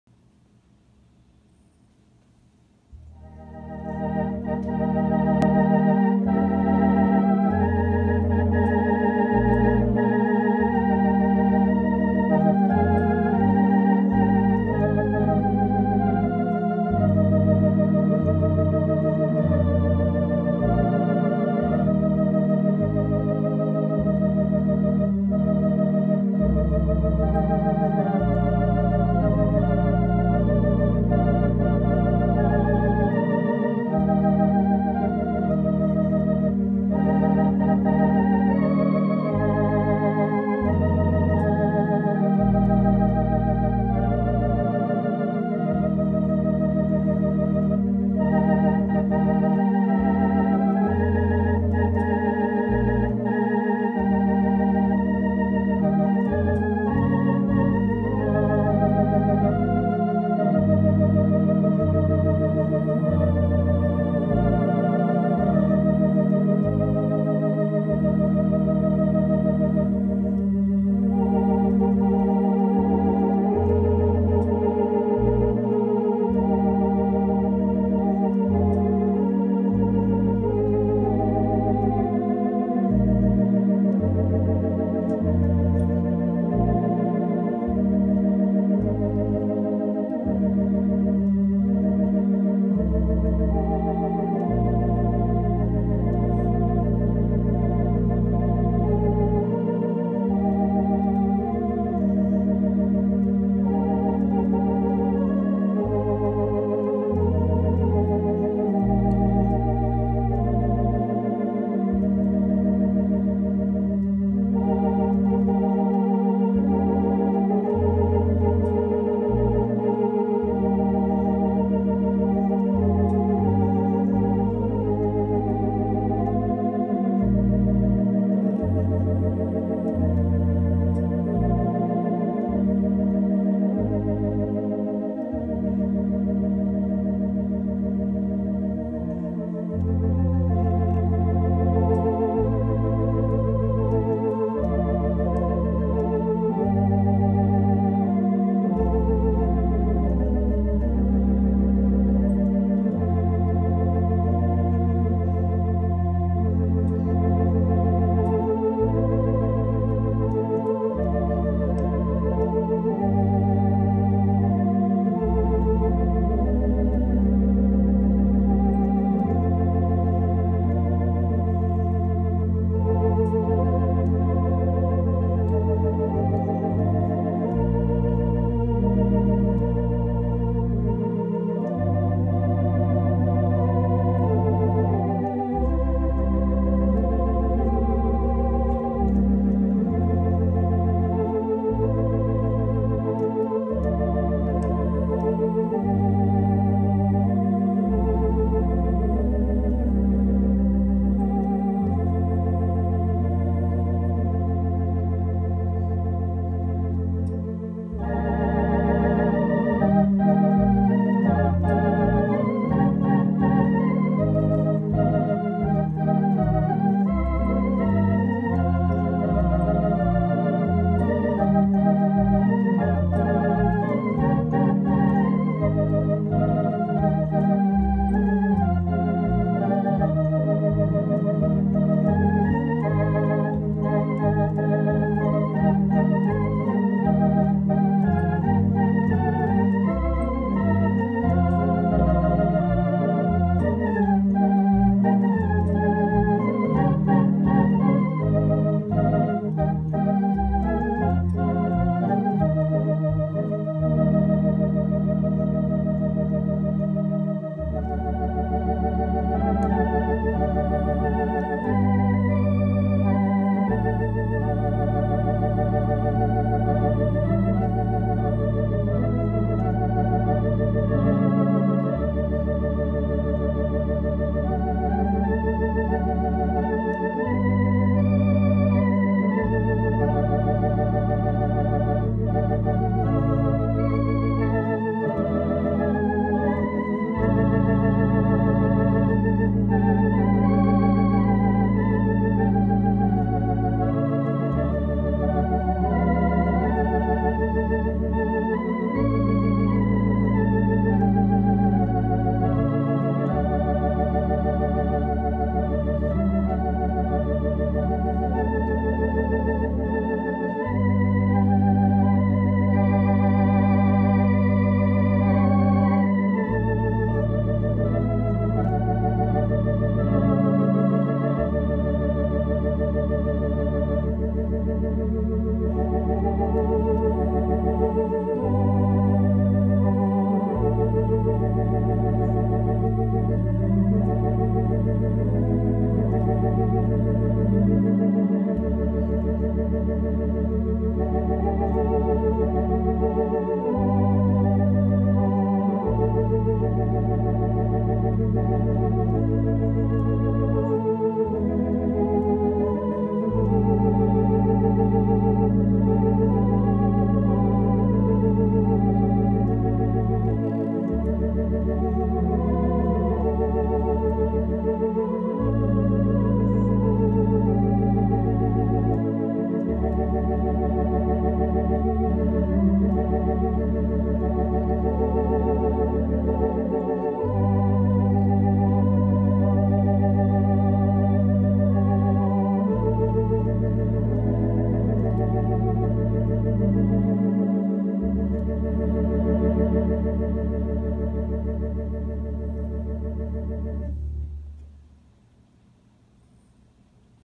Medley, by Calvin Yawn on Orga-sonic
Just me playing a few songs on my 1965 Baldwin model 55A Orga-sonic organ.